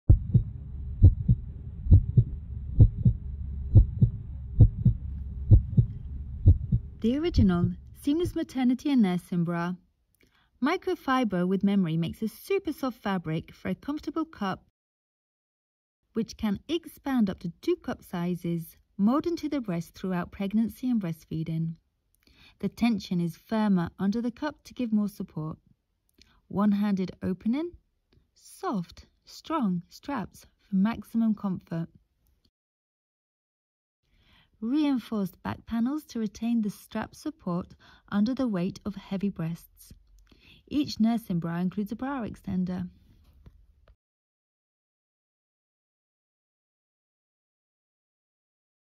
Carriwell Original Maternity & Nursing Bra
Introducing the Carriwell Original Maternity & Nursing Bra, the epitome of comfort and functionality for nursing mothers. This soft and comfortable bra is designed to adapt to your changing body shape, ensuring a perfect fit throughout your breastfeeding journey. Experience the most comfortable nursing bra you will ever wear!